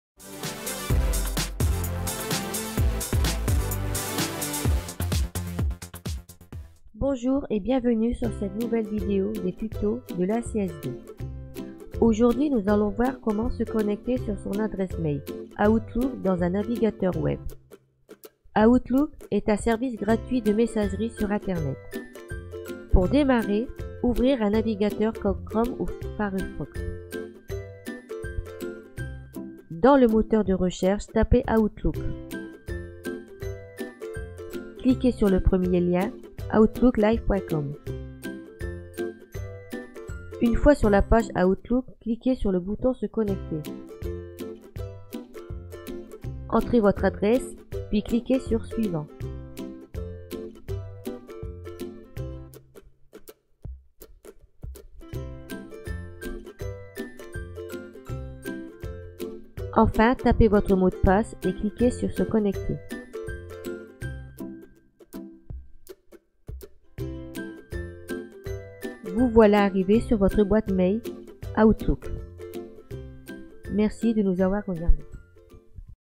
LES TUTOS DE L’ACSD – OUTLOOK – QUARTIERS#DOUAI3.0
Association des Centres Sociaux de Douai
Tuto réalisé dans le cadre du projet numérique quartiers#douai3.0
Vidéo réalisée par les adultes dans le cadre des ateliers numériques